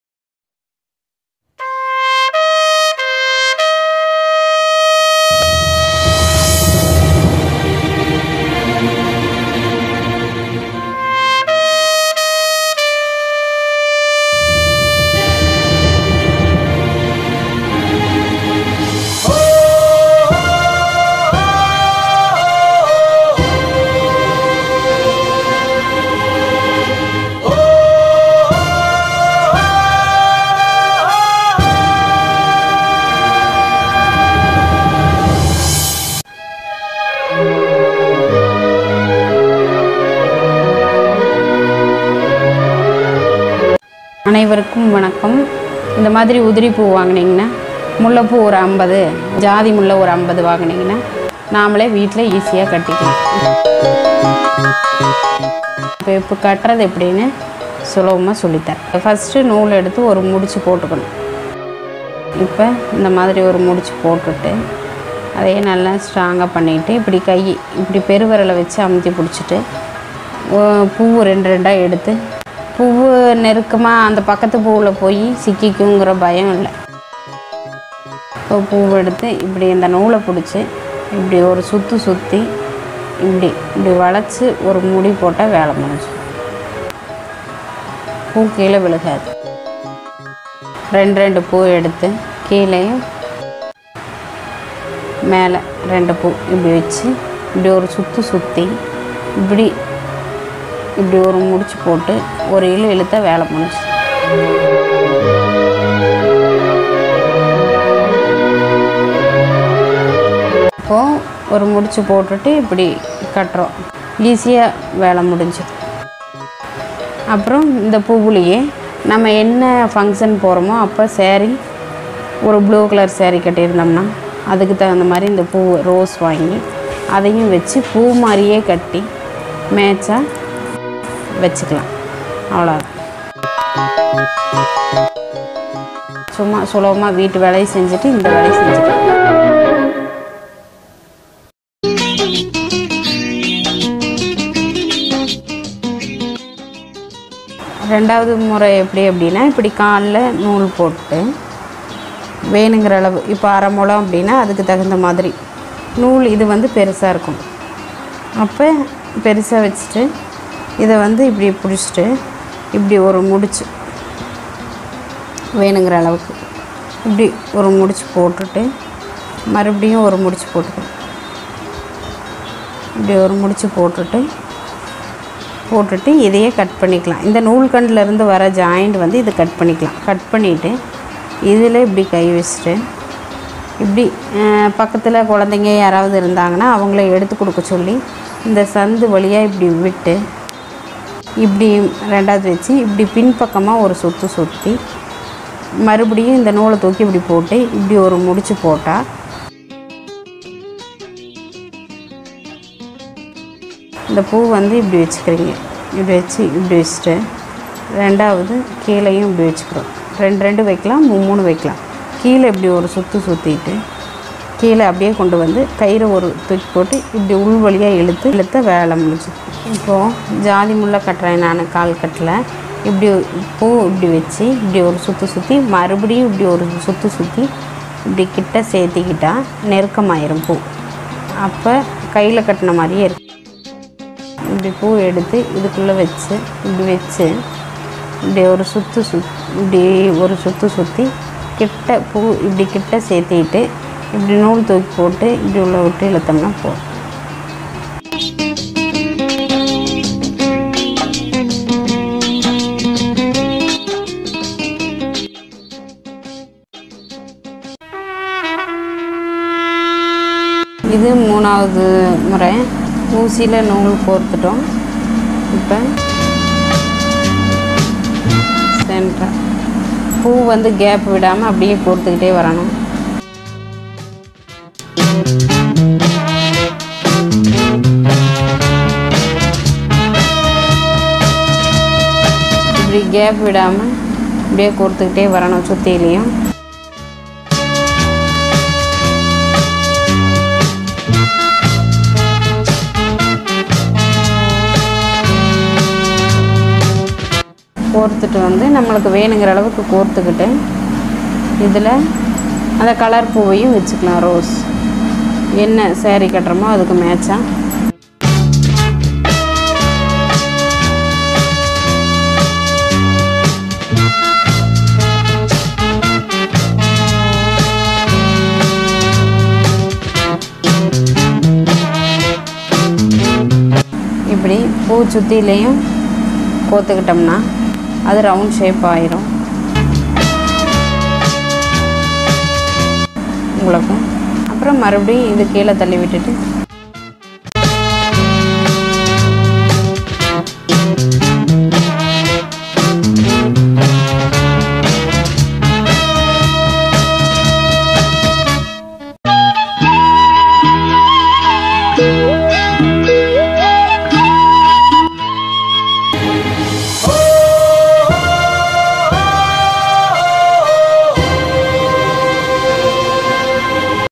Poo Kattuvathil Moondru Vagai /Megala/Farst video
Poo Kattuvathai Thelivaaga Virivaaga Intha Video moolam katrukkollalaam
3 vithamaana vagaiyil katrukkollalaam

pinnaniyil ilyaraja music